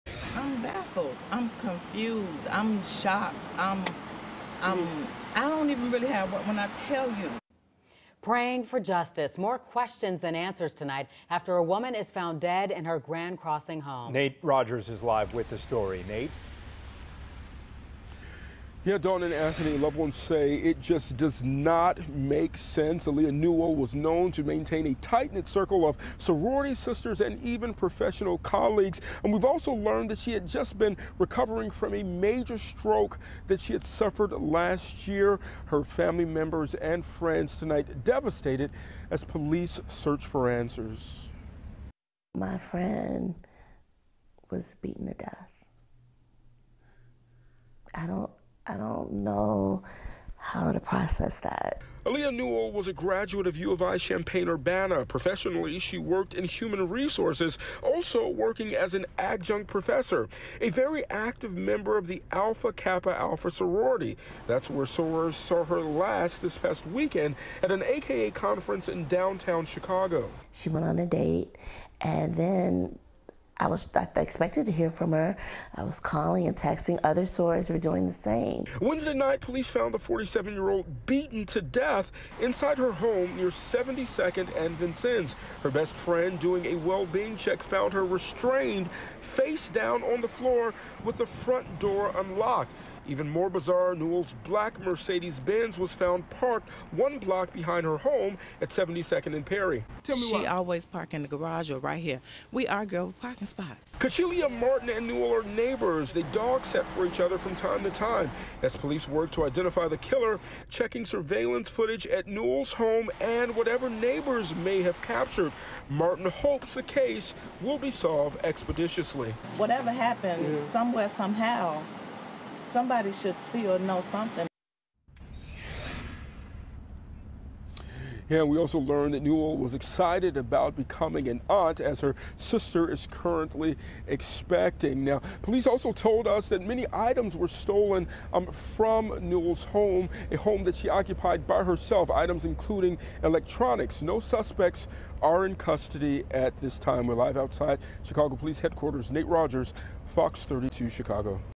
Aaliyah Newell murder: Chicago woman found beaten to death, restrained inside home
Close friends say that on Sunday, Aaliyah Newell met them for drinks and told them she had a date later in the evening. That was the last time she was seen alive.